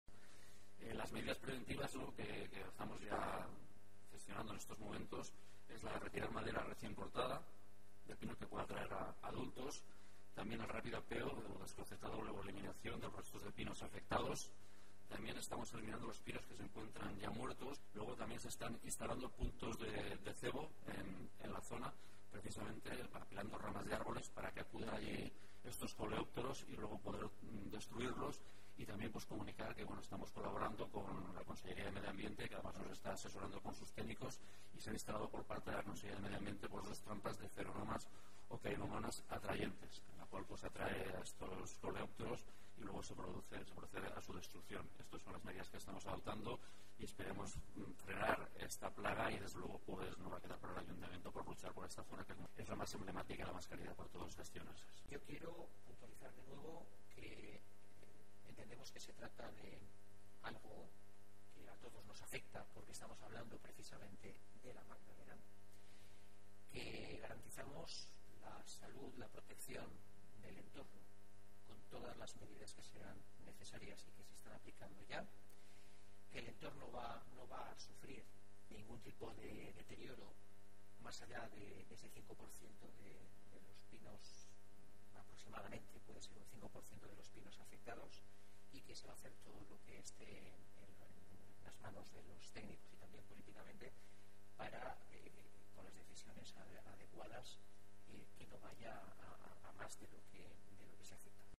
Intentan erradicar la plaga que afecta a pinos del entorno de la Magdalena
El portavoz del grupo municipal del PP, Juan José Pérez Macián, y el concejal de Sostenibilidad, Gonzalo Romero, han afirmado hoy que el Ayuntamiento de Castellón ya ha activado todas las medidas necesarias para erradicar la plaga que en estos momentos y debido a la sequía está afectando a pinos del entorno de la Magdalena. Así lo han explicado hoy ante los medios de comunicación y después de haber informado en la comisión informativa a los grupos de la oposición y en la que también ha intervenido el técnico municipal, quien ha explicado esta situación.